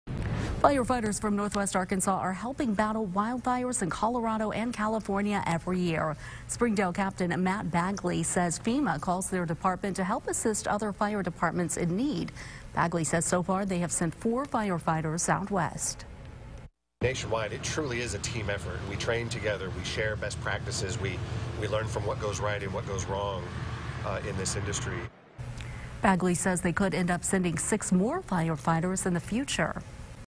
Springdale firefighters battling fires out west
Firefighters from Northwest Arkansas are helping battle wildfires in Colorado and California.